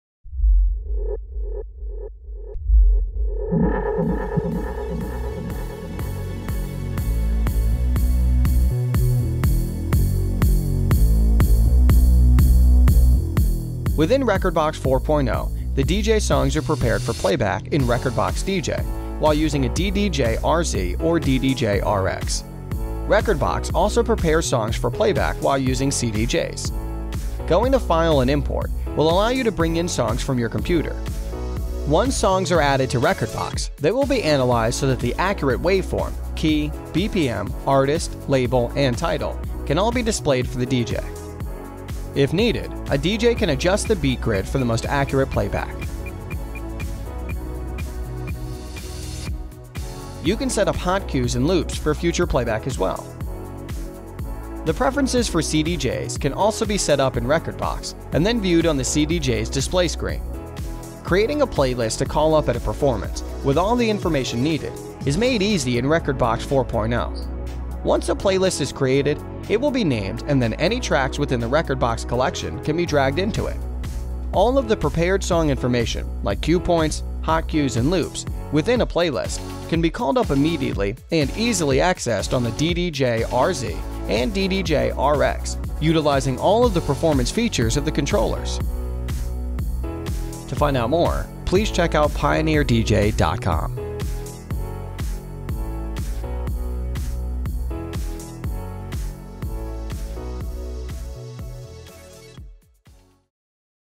rekordbox dj - Prepping Tracks & Creating Playlists
Learn about prepping tracks and creating playlists in rekordbox and rekordbox dj performance software.